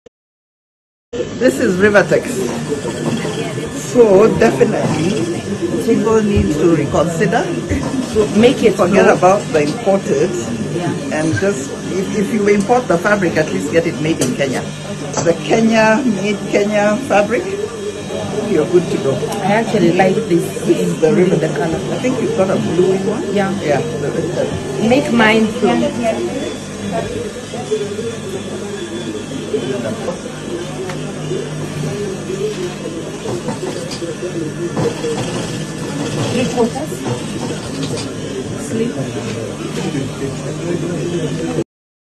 This is where Martha Karua gets her Beautiful Outfits | Rivatex Kenya
Martha Karua today African Kitenge dresses are designed by Flo Masinde at Rivatex Kenya. This comes after the Mitumba speech by Raila Odinga and DP Ruto. Kenya News Today, Citizen TV Live Now, KTN News and NTV Kenya Online livestream.